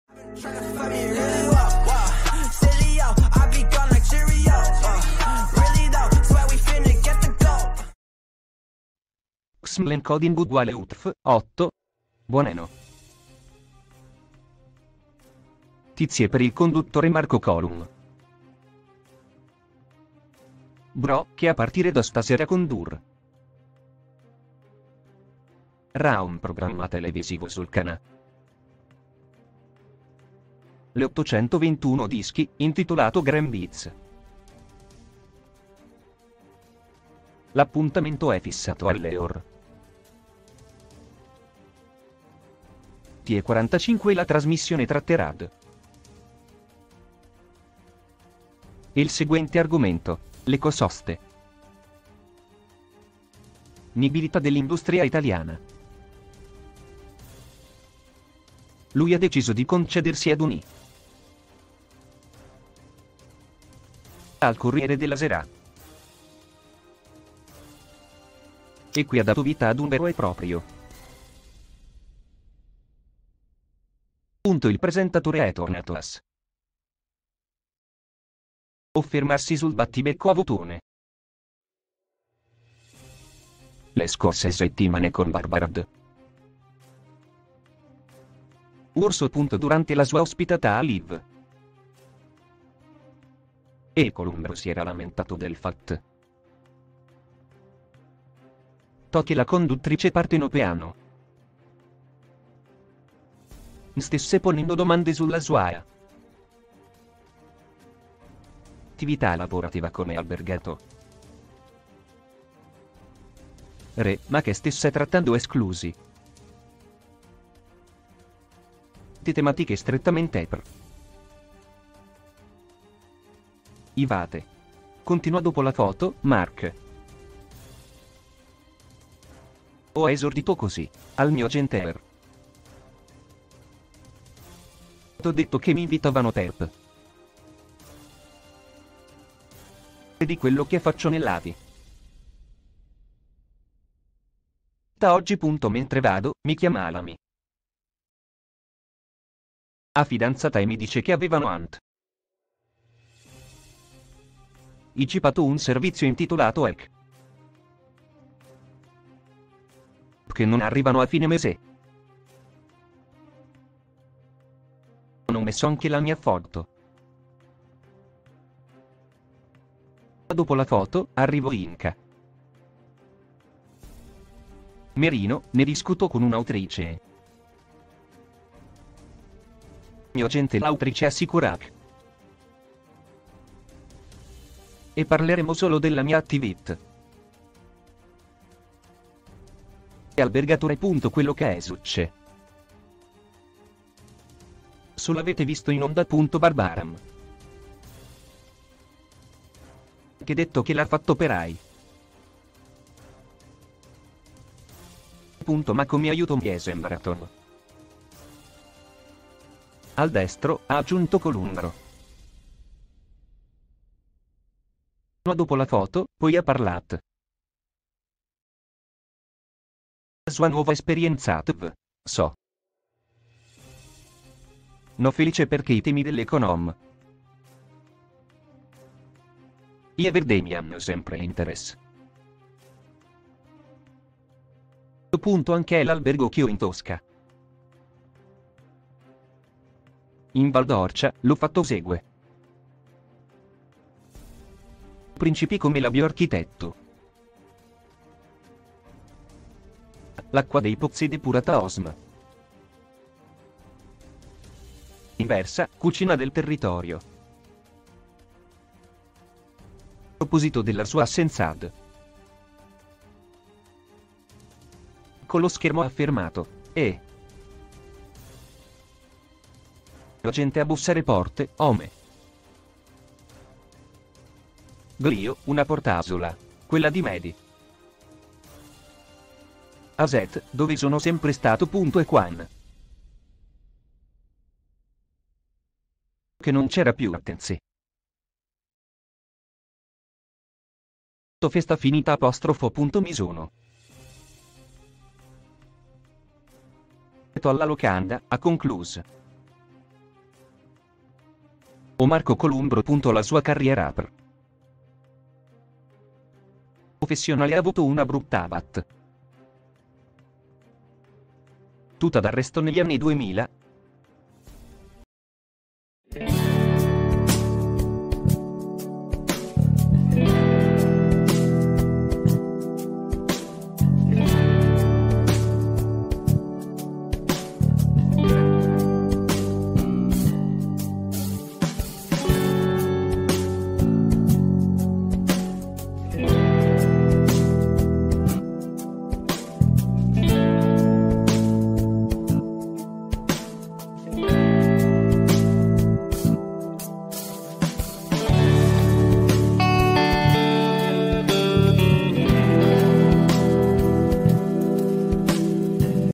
“Prima in camerino, poi lo avete visto tutti”. Marco Columbro parla: perché lo scontro con Barbara D
“Prima in camerino, poi lo avete visto tutti”. Marco Columbro parla: perché lo scontro con Barbara D’Urso